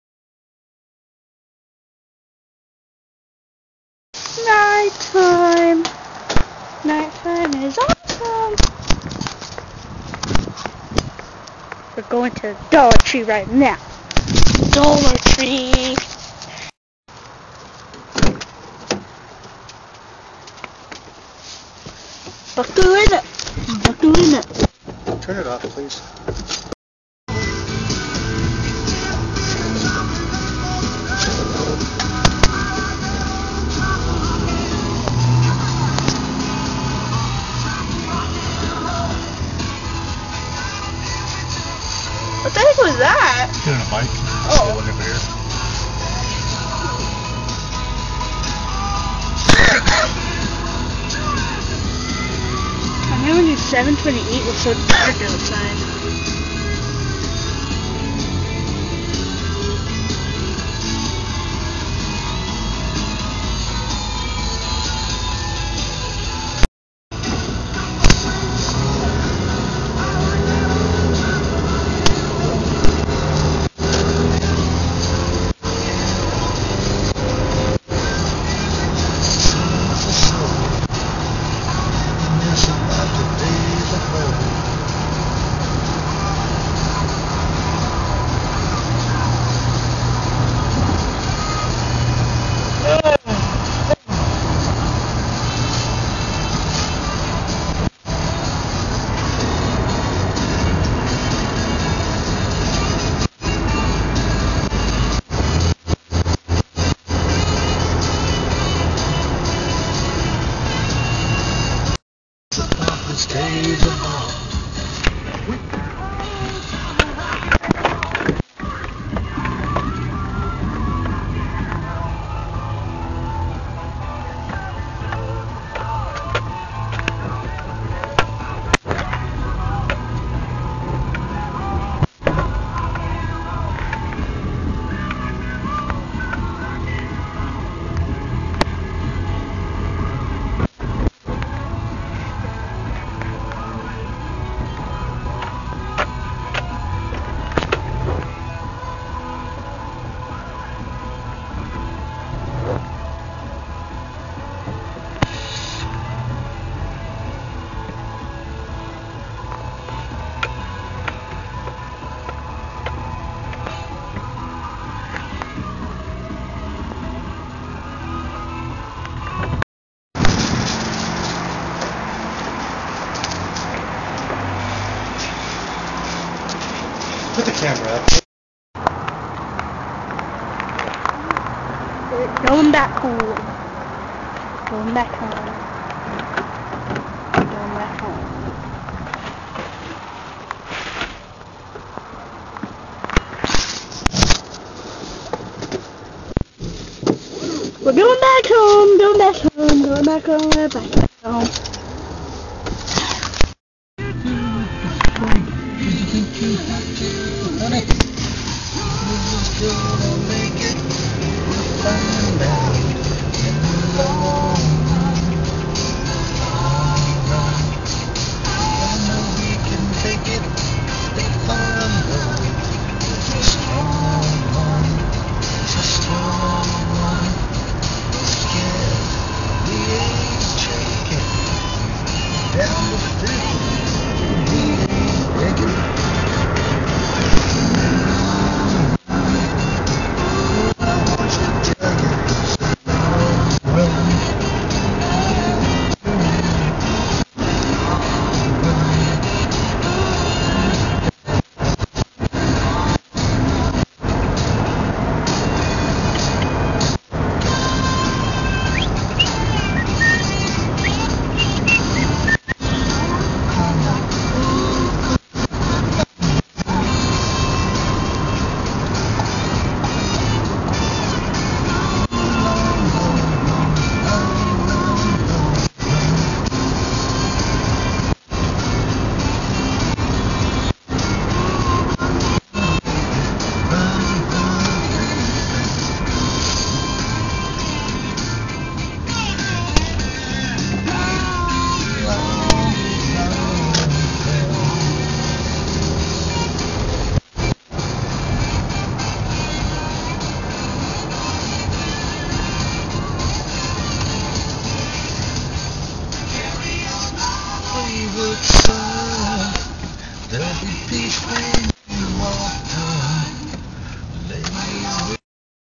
journey to dollar tree
Me and my dad go to dollar tree at like 7:30 at night